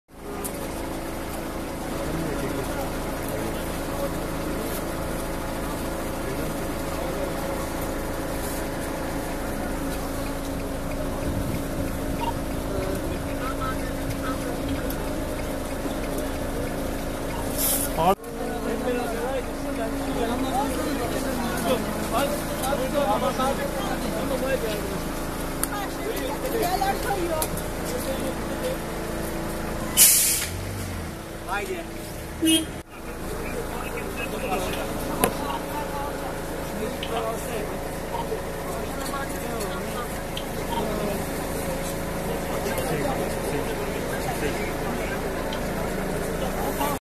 Motosiklet park halindeki kamyonete çarptı:1 yaralı
Bartın'da motosiklet park halindeki kamyonete çarptı:1 yaralı